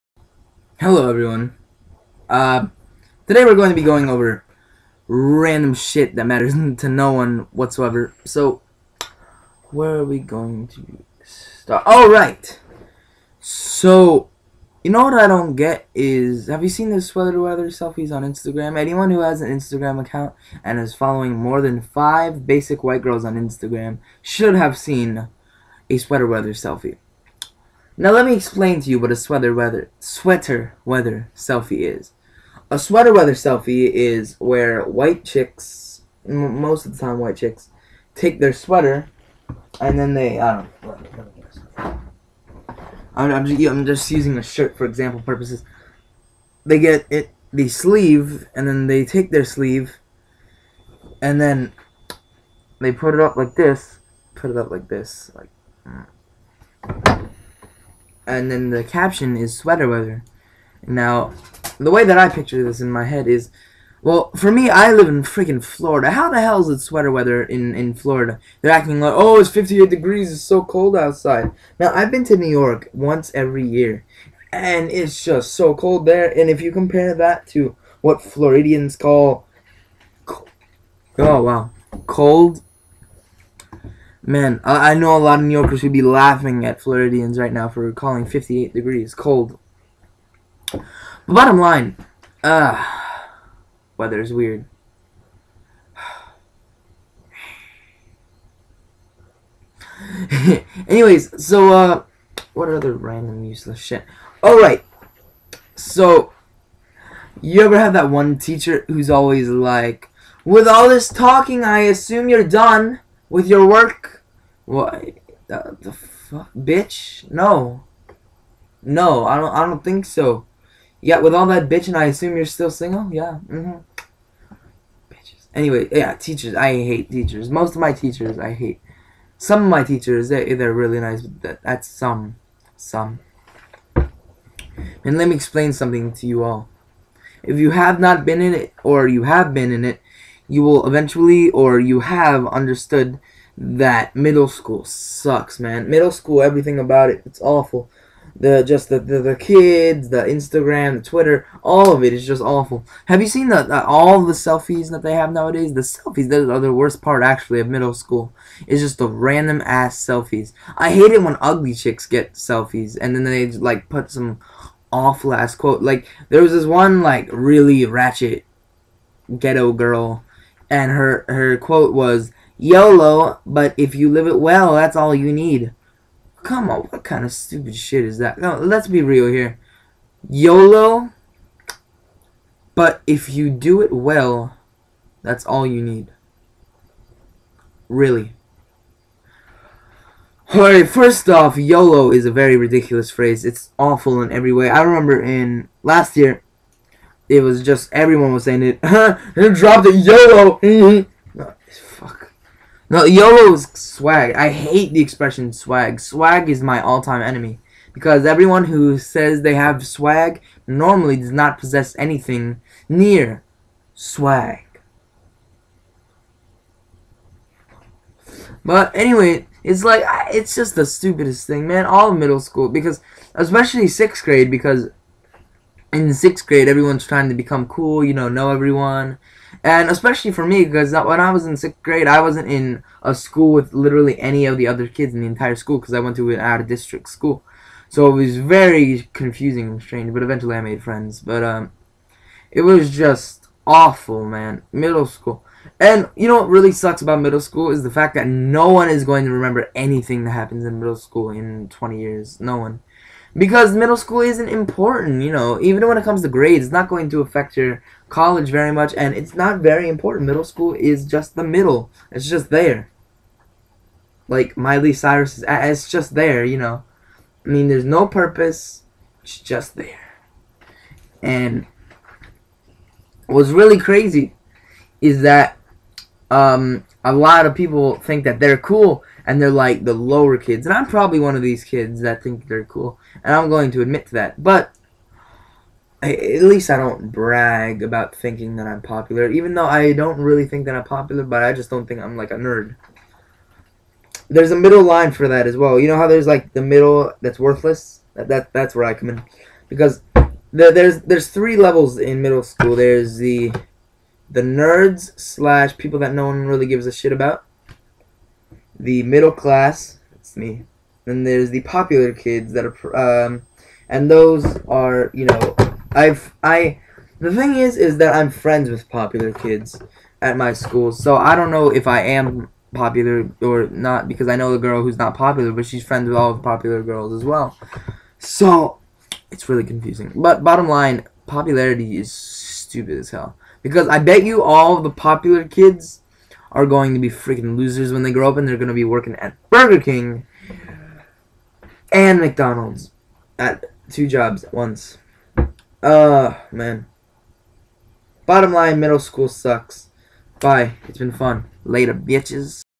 Sweater Weather, Teachers, And Middle School
Ranting about Sweater Weather Selfies, Teachers, Middle School, and More! Thanks!